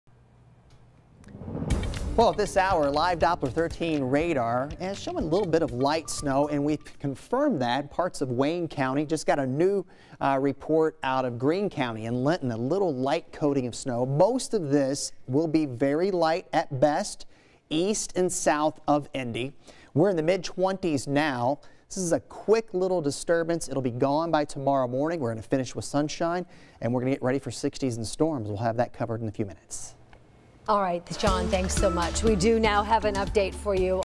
Southern Indiana to see some snow Tuesday night
Sean Ash has the latest.